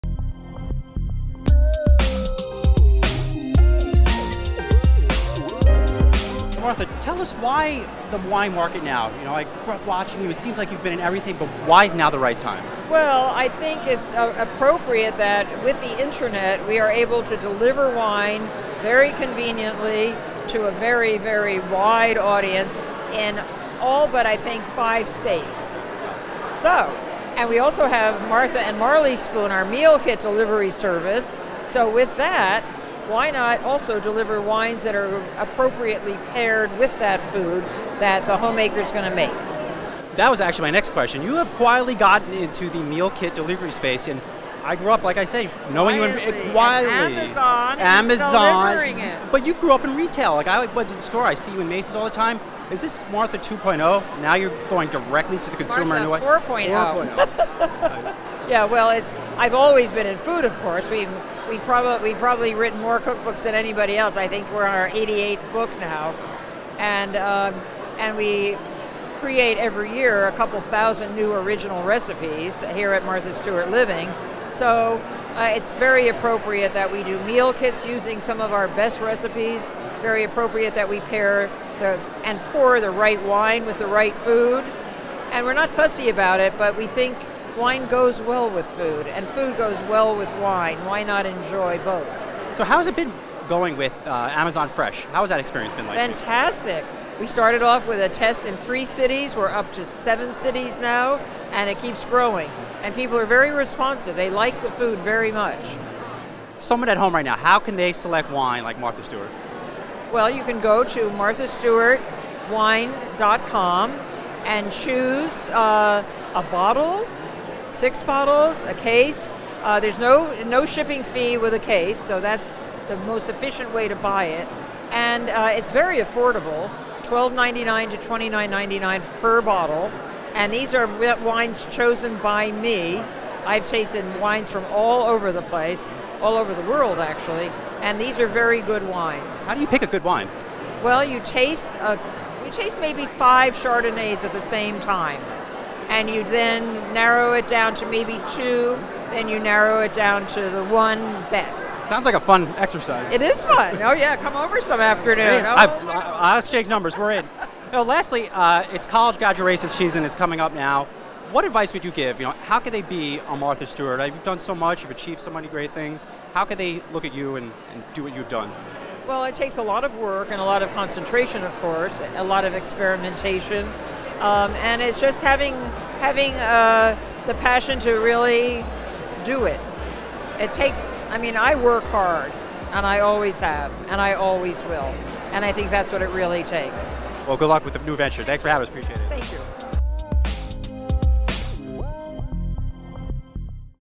Martha Stewart Perfectly Explains Why She's Is Now Selling Wine on the Internet
And you thought Martha Stewart was done finding new areas to extend her well-known brand beyond the shelves of Macy's and Home Depot  (and countless other areas). The domestic diva has just entered the wine market via the launch of Martha Stewart Wine Co.

The venture aims to sell wine fans a collection of wines each month that are personally picked by Stewart. They are then delivered straight to a person's house. Or, the wine could be purchased a la carte.

TheStreet talked with Stewart about her newest venture.